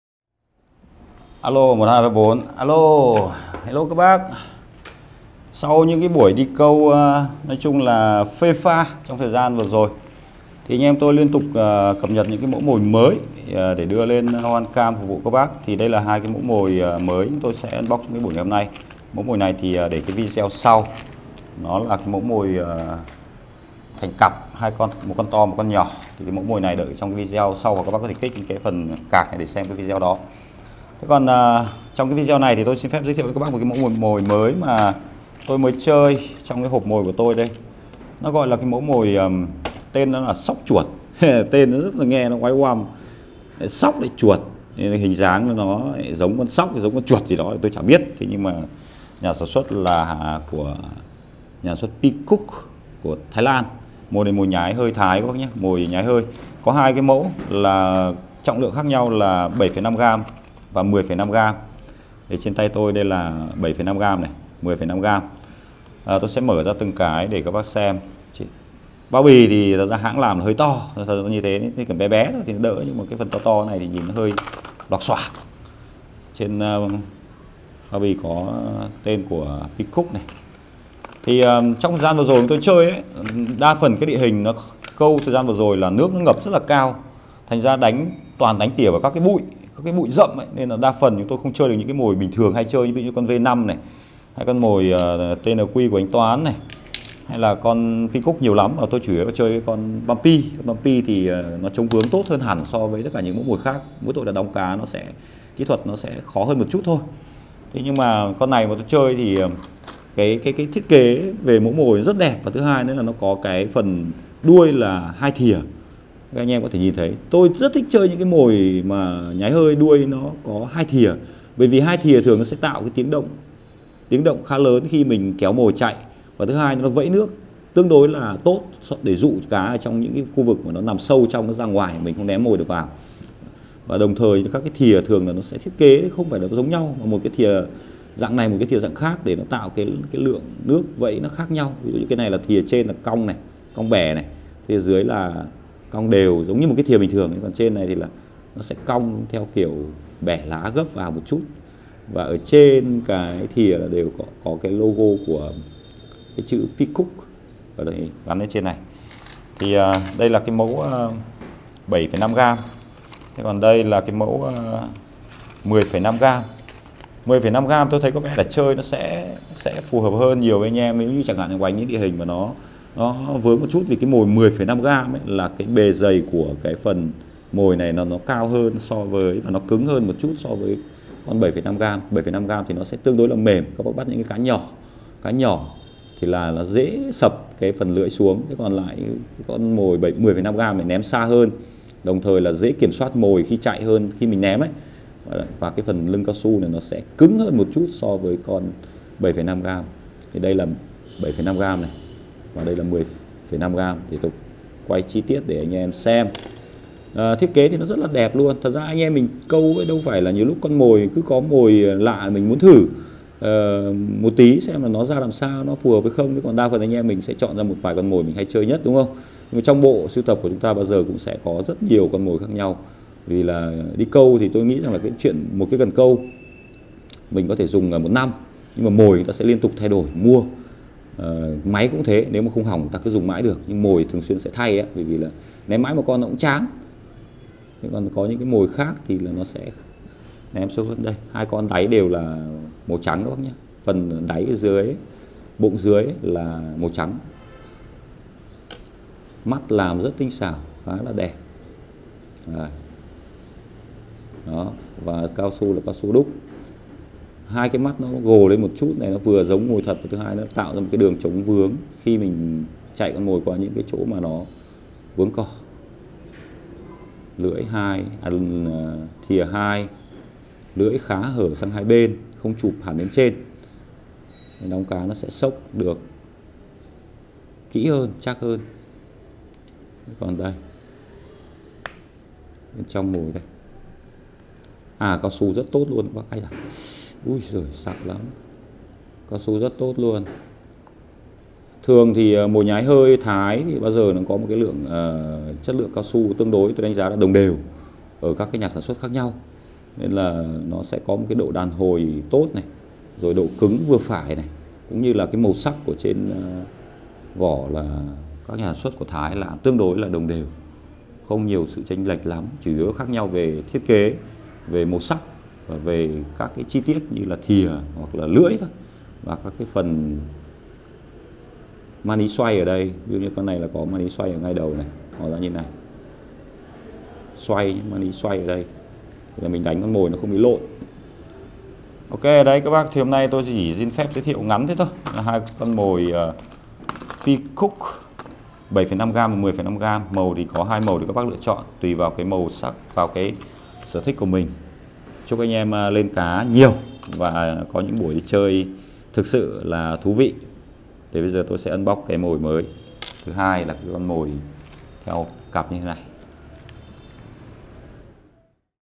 MỒI LURE SÓC CHUỘT
Mồi Lure Nhái Hơi – Sóc Chuột thích hợp để câu lure, câu cá lóc, cá chuối sộp… Sở hữu Action đẹp kết hợp 2 thìa xoay tạo bọt thu hút cá cực tốt.

● HOA BAN CAMP™ - Nhà số 7, Khu liền kề Minori, 67A Trương Định, Quận Hai Bà Trưng, Hà Nội
● Điện thoại tư vấn & đặt hàng:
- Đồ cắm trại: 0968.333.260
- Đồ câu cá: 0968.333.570